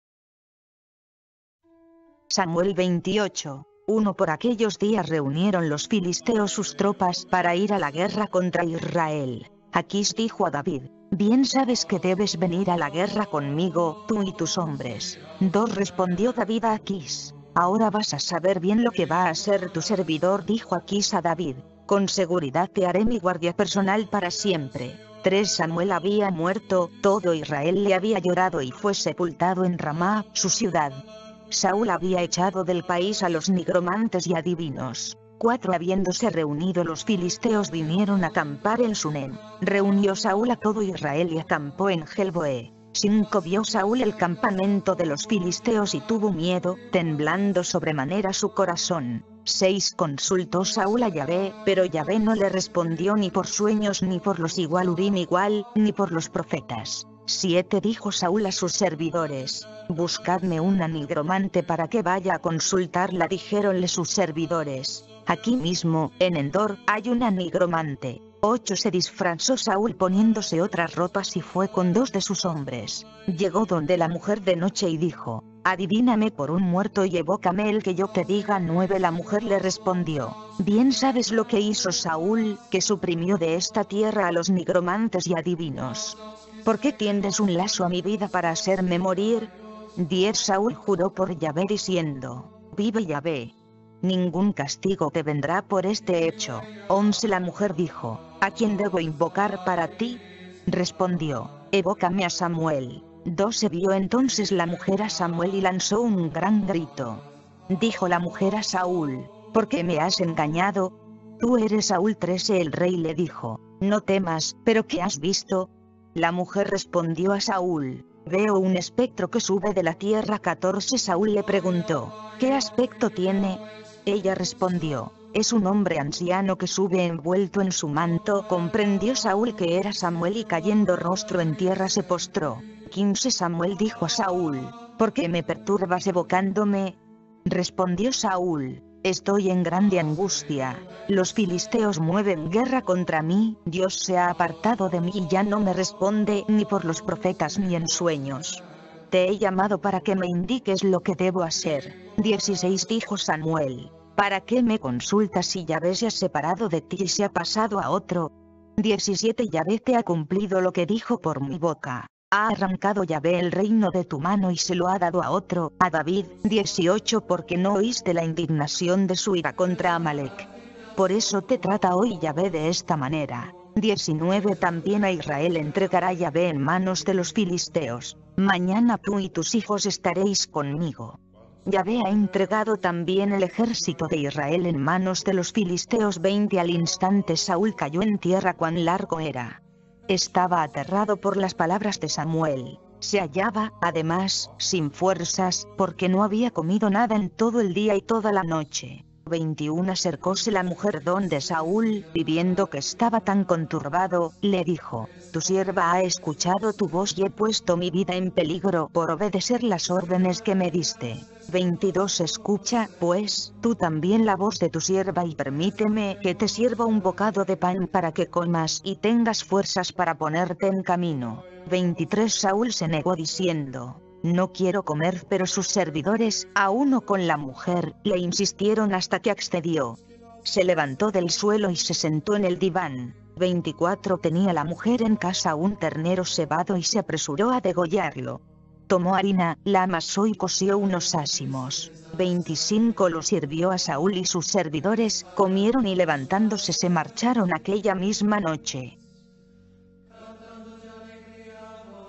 LA SANTA BIBLIA,VERSIÓN BIBLIA DE JERUSALÉN 1976, 1 Samuel 28
Samuel 28

1 Por aquellos días reunieron los filisteos sus tropas para ir a la guerra
contra Israel; Akís dijo a David: «Bien sabes que debes venir a la guerra
conmigo, tú y tus hombres.»
2 Respondió David a Akís: «Ahora vas a saber bien lo que va a hacer
tu servidor.» Dijo Akís a David: «Con seguridad te haré mi guardia
personal para siempre.»
3 Samuel había muerto, todo Israel le había llorado y fue sepultado en
Ramá, su ciudad. Saúl había echado del país a los nigromantes y adivinos.
4 Habiéndose reunido los filisteos vinieron a acampar en Sunem.
Reunió Saúl a todo Israel y acampó en Gelboé.
5 Vio Saúl el campamento de los filisteos y tuvo miedo, temblando
sobremanera su corazón.
6 Consultó Saúl a Yahveh, pero Yahveh no le respondió ni por sueños
ni por los = urim =, ni por los profetas.
7 Dijo Saúl a sus servidores: «Buscadme una nigromante para que
vaya a consultarla.» Dijéronle sus servidores: « Aquí mismo, en Endor, hay
una nigromante.»
8 Se disfrazó Saúl poniéndose otras ropas y fue con dos de sus
hombres; llegó donde la mujer de noche y dijo: «Adivíname por un muerto
y evócame el que yo te diga.»
9 La mujer le respondió: «Bien sabes lo que hizo Saúl, que suprimió
de esta tierra a los nigromantes y adivinos. ¿Por qué tiendes un lazo a mi
vida para hacerme morir?»
10 Saúl juró por Yahveh diciendo: «¡Vive Yahveh! Ningún castigo te
vendrá por este hecho.»
11 La mujer dijo: «¿A quién debo invocar para ti?» Respondió:
«Evócame a Samuel.»
12 Vio entonces la mujer a Samuel y lanzó un gran grito. Dijo la
mujer a Saúl: «¿Por qué me has engañado? ¡Tú eres Saúl¡»
13 El rey le dijo: «No temas, pero ¿qué has visto?» La mujer
respondió a Saúl: «Veo un espectro que sube de la tierra.»
14 Saúl le preguntó: «¿Qué aspecto tiene?» Ella respondió: «Es un
hombre anciano que sube envuelto en su manto.» Comprendió Saúl que era
Samuel y cayendo rostro en tierra se postró.
15 Samuel dijo a Saúl: «¿Por qué me perturbas evocándome?»
Respondió Saúl: «Estoy en grande angustia; los filisteos mueven guerra
contra mí, Dios se ha apartado de mí y ya no me responde ni por los
profetas ni en sueños. Te he llamado para que me indiques lo que debo
hacer.»
16 Dijo Samuel: «¿Para qué me consultas si Yahveh se ha separado de
ti y se ha pasado a otro?